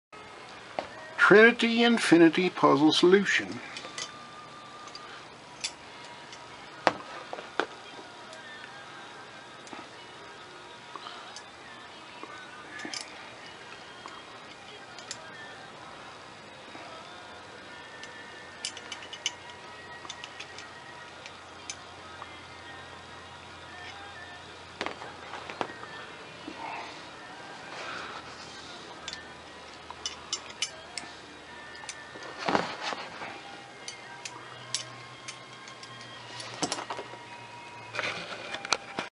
Solution for Trinity Infinity from Puzzle Master Metal Puzzles
If you would like to get more solutions to puzzles like this or any other puzzle go

If you are looking for a solution to Trinity Infinity from Puzzle Master look no further. Take the 3 metal pieces apart and put them back to gether again.   Get step by step instructions on how to solve this puzzle from Puzzle Master.

for more great puzzles.

Thank you to Eureka Puzzles for giving us this solution.